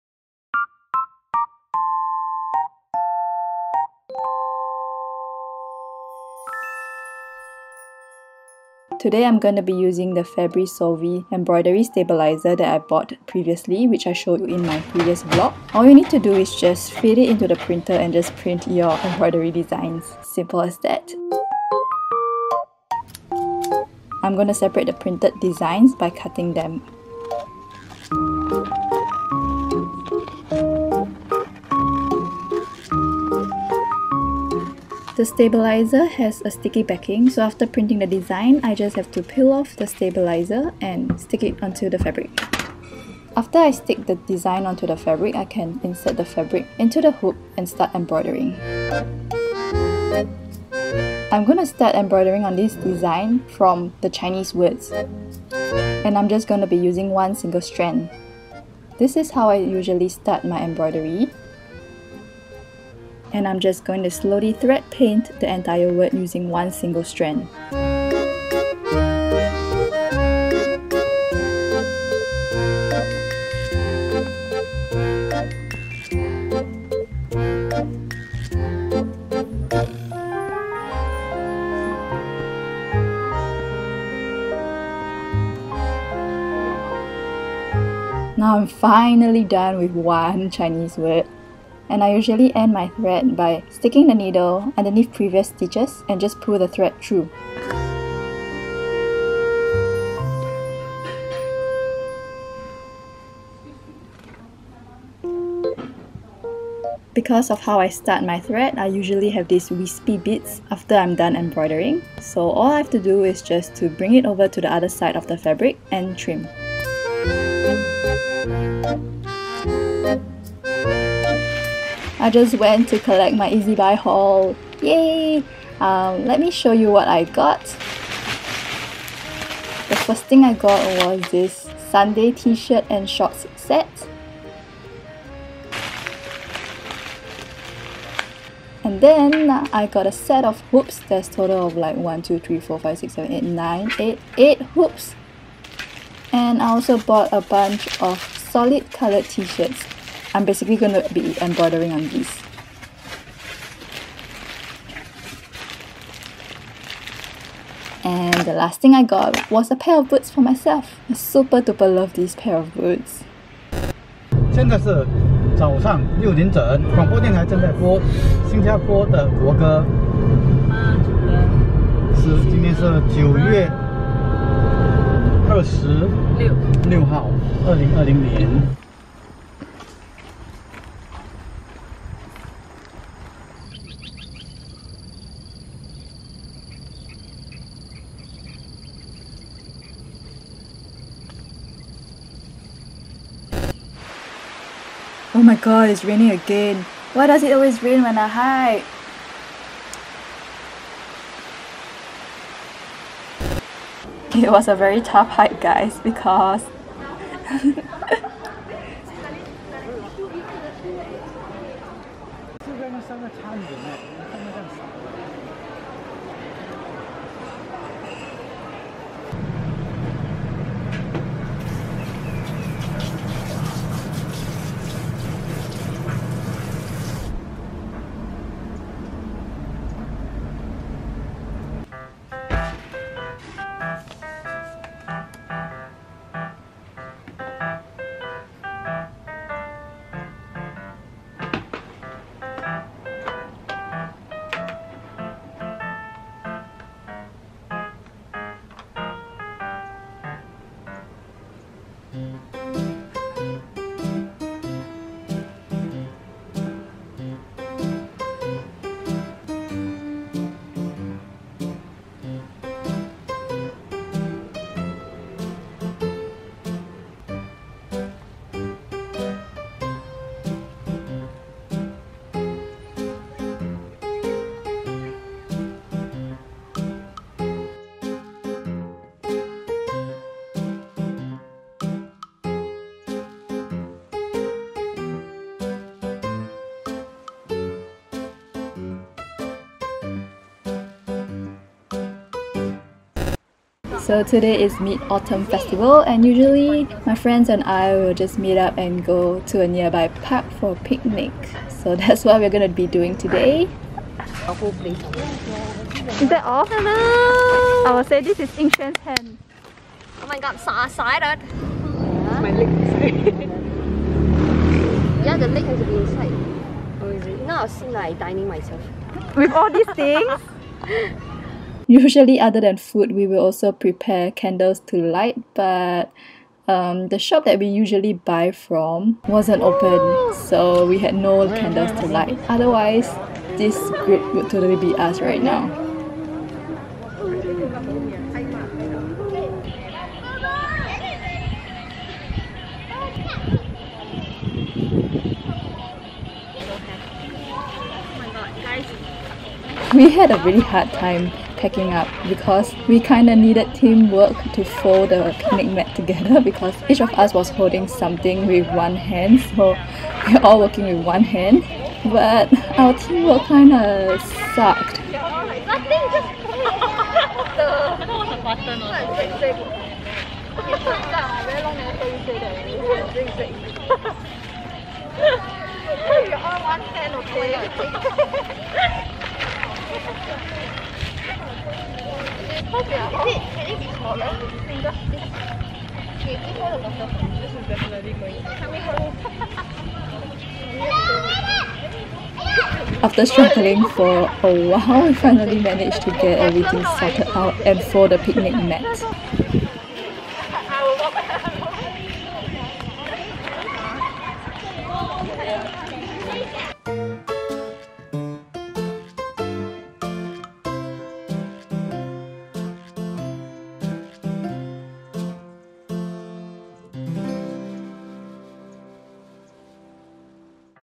[ENG CC] a homebody's day | The Untamed 🧵 embroidery, 🛒 taobao haul, 🧺 picnic | vlog #22
Please turn on CC before watching~ thank you!

This was filmed back in September! Afterwards I got a little busy so didn't manage to edit it until December (I also stopped filming during this period haha). It was my first time using the Solvy stabiliser and I really love how convenient it is! Also, it seems like it always rains when I hike hahaha. Maybe I'm not meant to hike :P

00:00 snippets
00:08 Solvy stabiliser (embroidery)
02:01 Taobao/ezbuy haul
02:45 hiking
03:33 lunch!
03:40 washing my shoes
03:52 continue embroidery
04:54 mid-autumn picnic